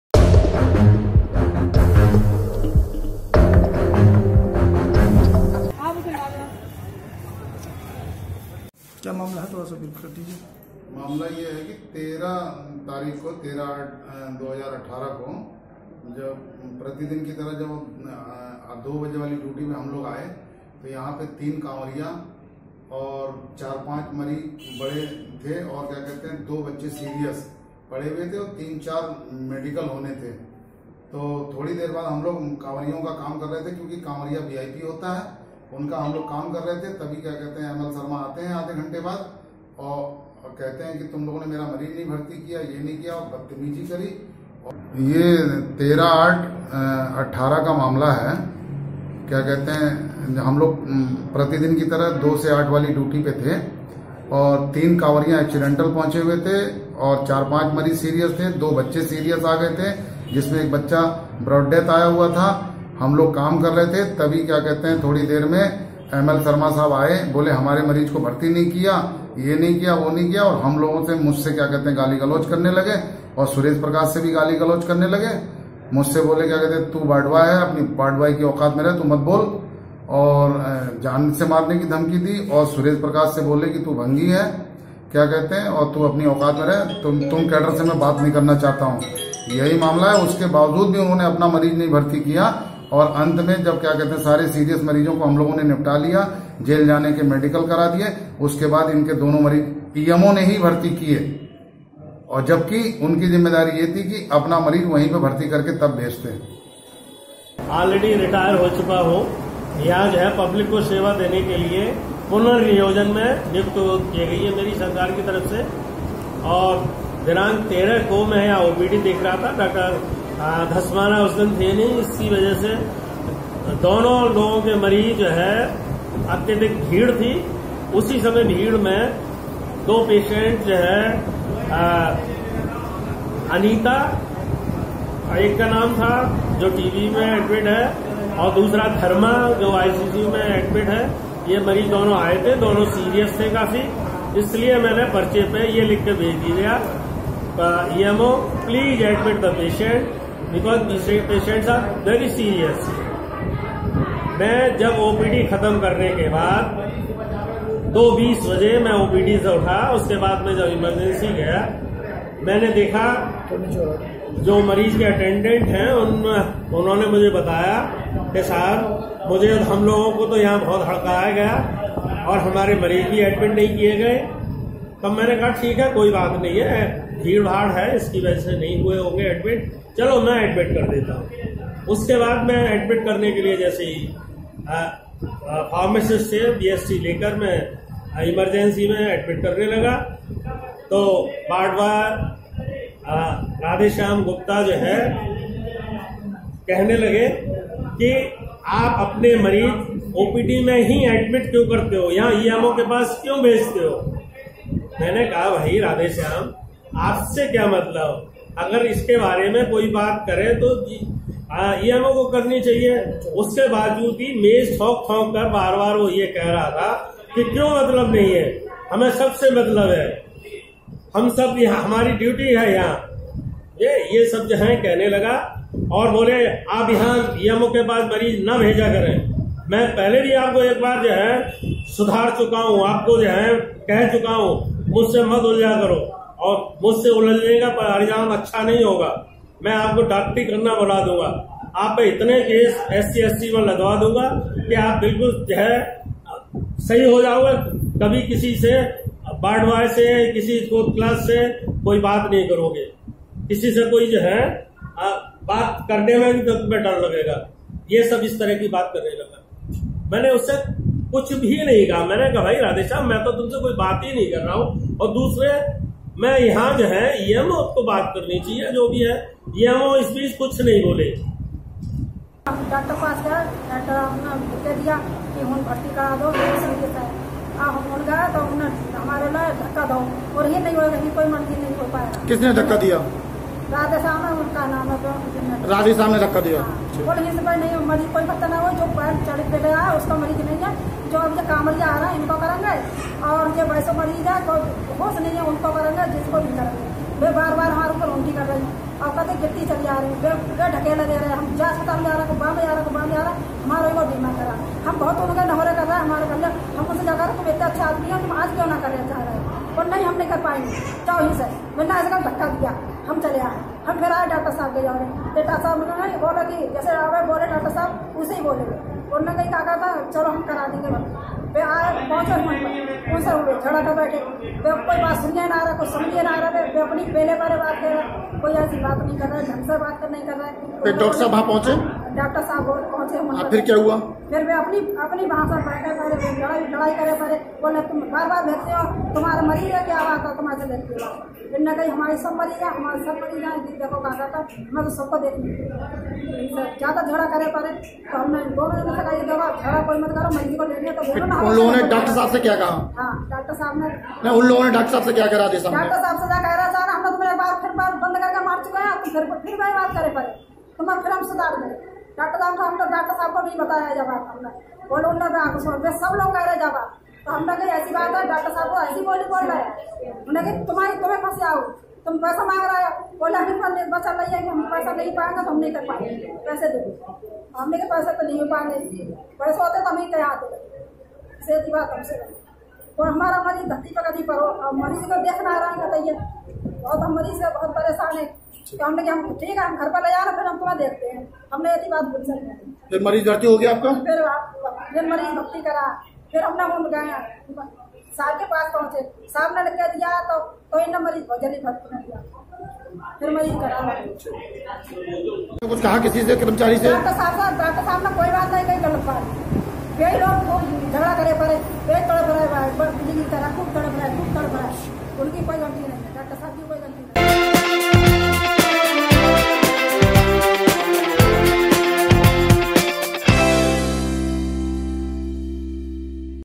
Bareilly News : डॉक्टर और कर्मचारी में नोकझोक दोनो ने कोतवाली में दी तहरीर
बरेली।जिला अस्पताल के डॉक्टर एम एल शर्मा औरवार्ड बॉय राधे श्याम के बीच १३अगस्त को मरीज़ भर्ती को   लेकर काफी गरम गरमी हो गई थी और डॉक्टर एम एल शर्मा ने कोतवाली मे राधे श्याम के खिलाफ तहरीर थी है दूसरी पक्ष राधे श्याम न भी कोतवाली में डॉक्टर एम एल  शर्मा के खिलाफ जाती सूचक शब्द का प्रयोग करने और कर्म चारी को धमकाने  का आरोप लगाया है । पुलिस के पास दोनों पक्षों की तहरीर पहुंच गई है और जांच चल रही है